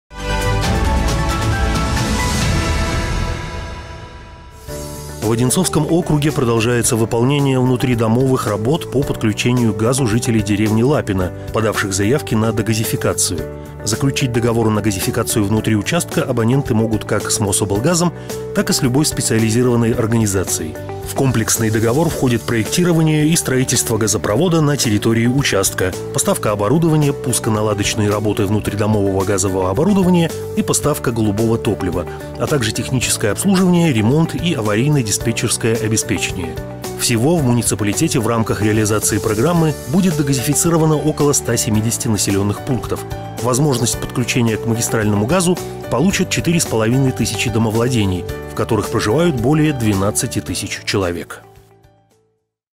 Соцгазификация в Лапине
В рамках соцгазификации продолжается подключение к газу домов в деревне Лапино. Всего возможность подключения к магистральному газу в муниципалитете получат 4,5 тысячи домовладений.